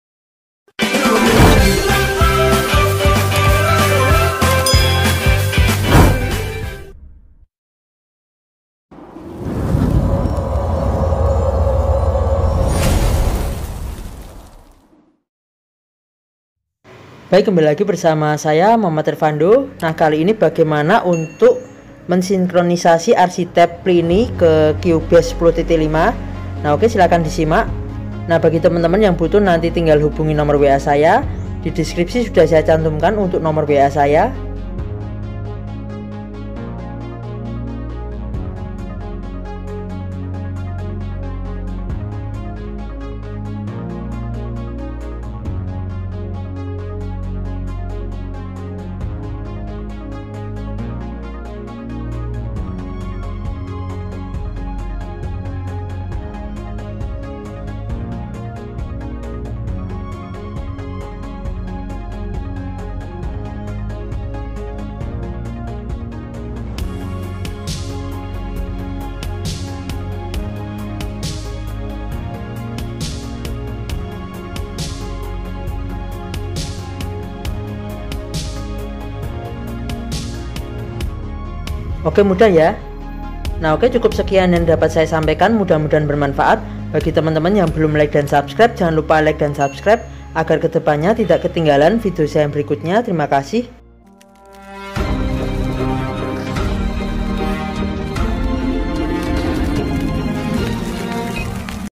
Neural DSP ARCHETYPE PLINI on Cubase 10.5
Video ini menjelaskan tentang bagaimana menyinkronkan Neural DSP ke Cubase 10.5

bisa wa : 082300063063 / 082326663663